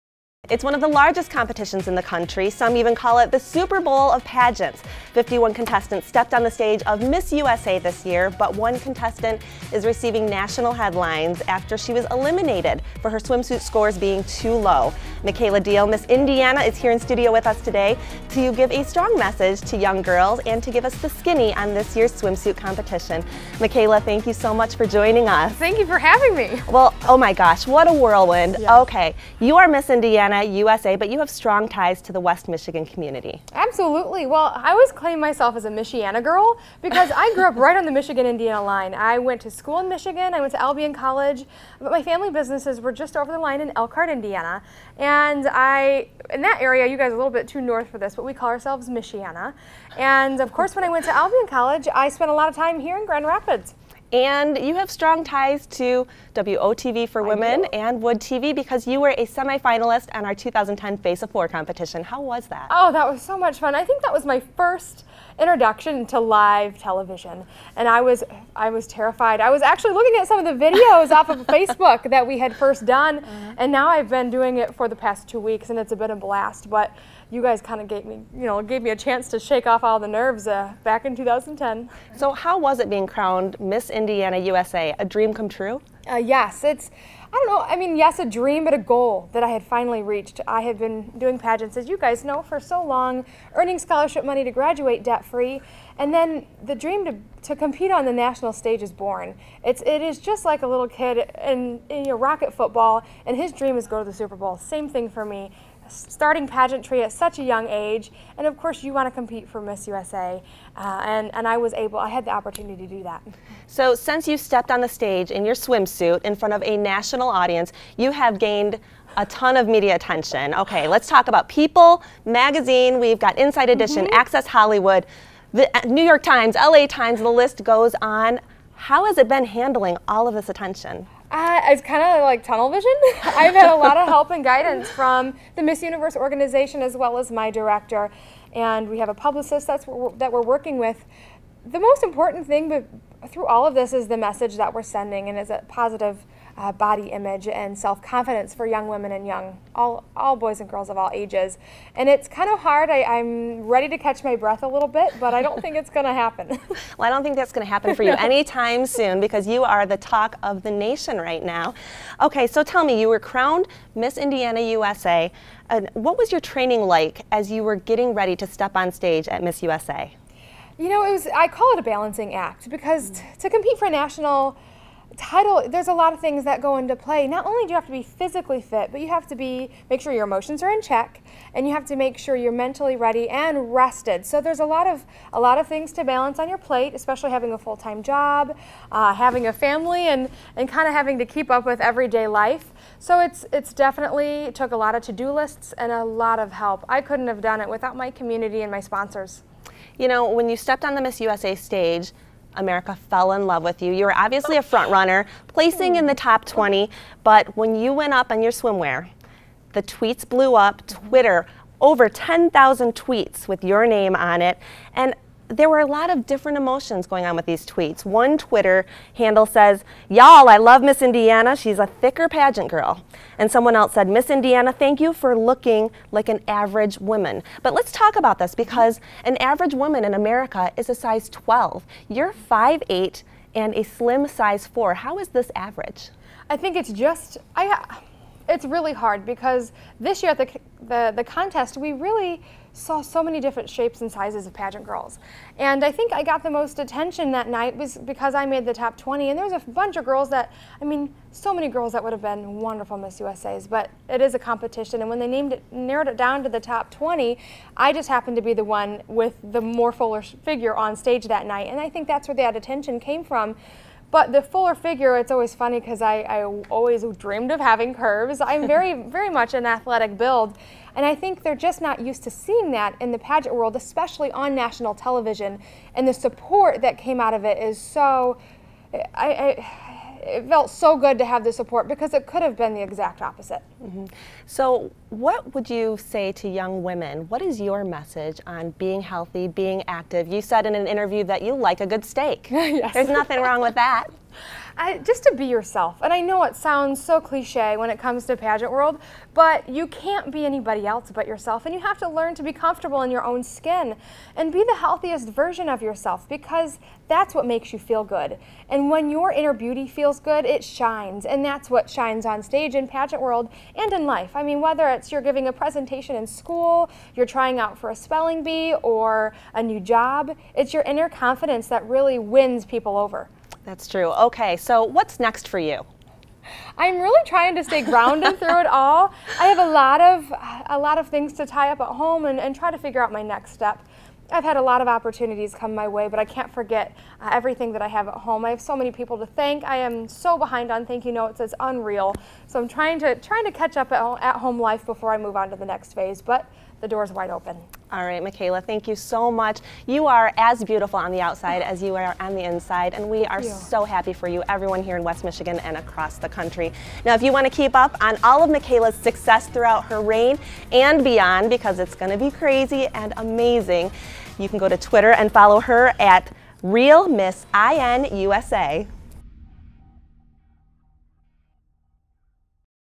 The Healthy Weight Debate with Miss Indiana USA
Jordan Carson sits down with Miss Indiana USA, Mekayla Diehl, for the healthy weight debate. Deihl's normal figure made national headlines after she was eliminated for having low swimsuit scores during the competition.